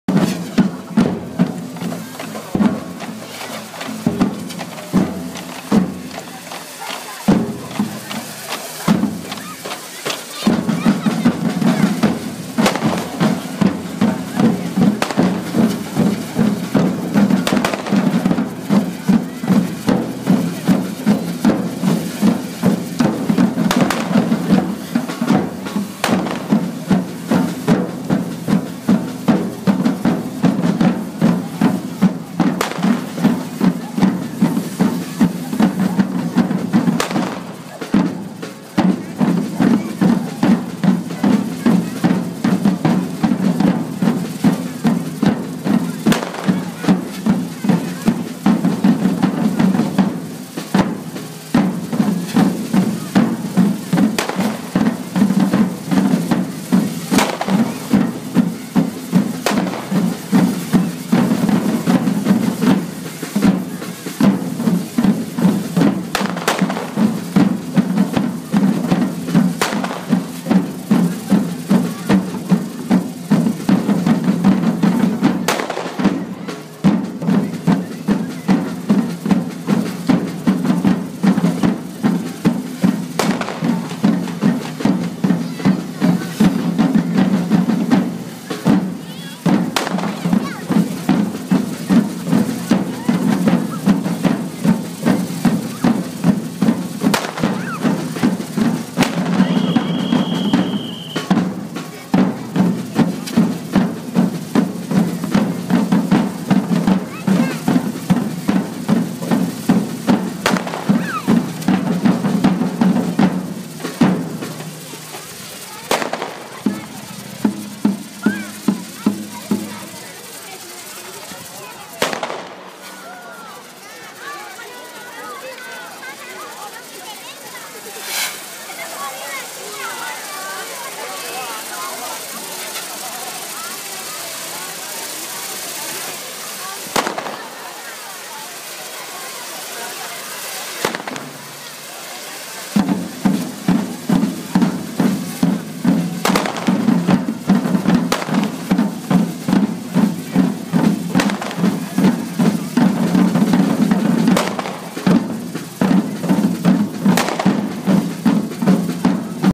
Enterro de SM Carnestoltes 2014
Els diables de Cubelles van protagonitzar la incineració de SM Carnestoltes